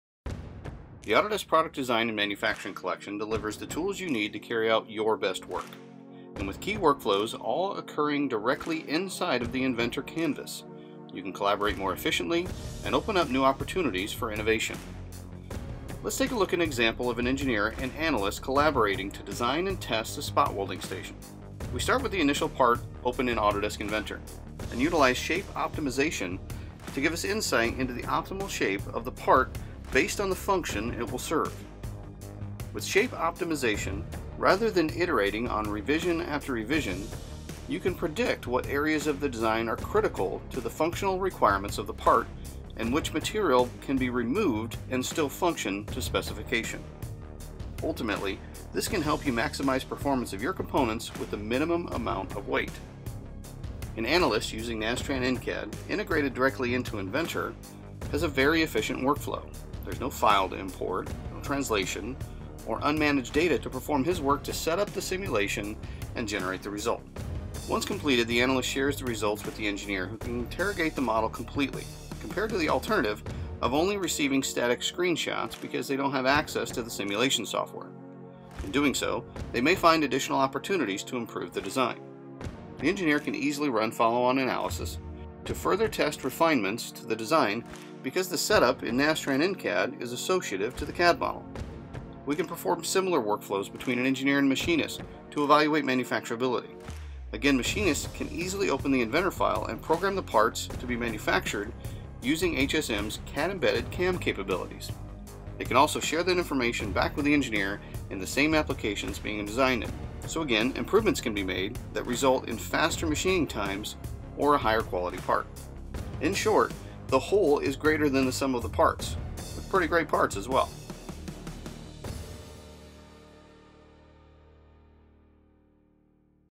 Product Design and Manufacturing Collection - Professional Grade 3D CAD
The Product Design and Manufacturing Collection now includes Autodesk Inventor, Nastran In-CAD, and Inventor HSM. Professional grade 3D CAD, Advanced non-linear simulation, and 5-Axis Milling.